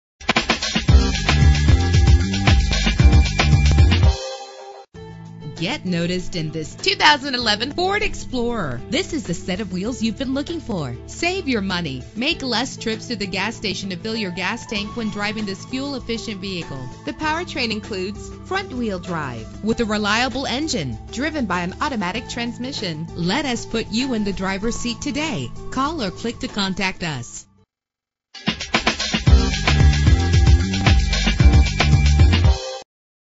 2011 Ford Explorer - Sheffield OH
Year: 2011
Make: Ford
Model: Explorer
Trim: Limited Edition
Engine: 3.5 L, 6 cylinder
Transmission:
Color: Black
Mileage: 49728
Address:
5050 Lorain Rd